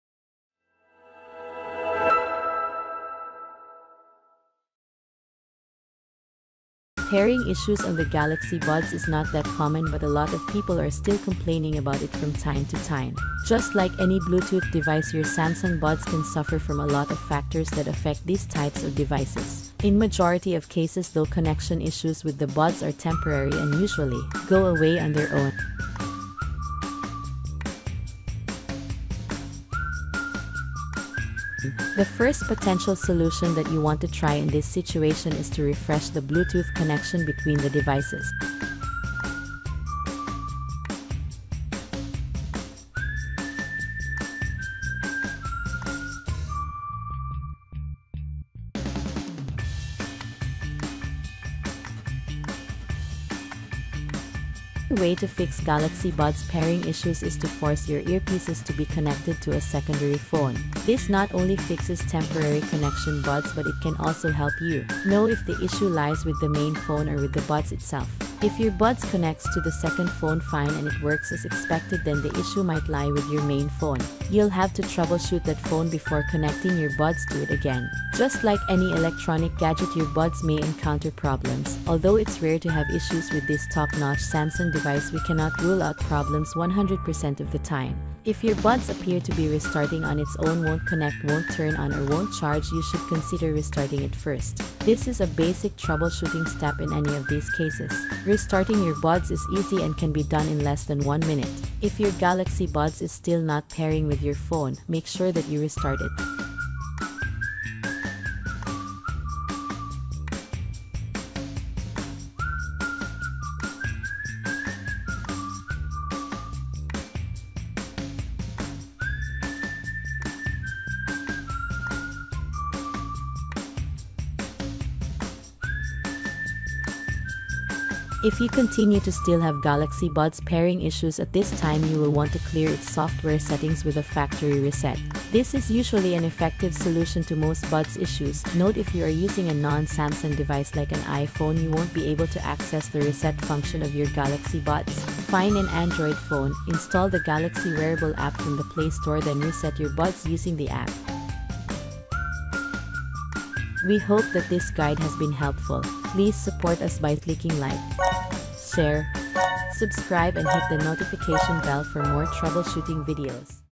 How To Fix Galaxy Buds Pairing Issues
You can try to fix your Samsung Galaxy Buds that is experiencing pairing issues.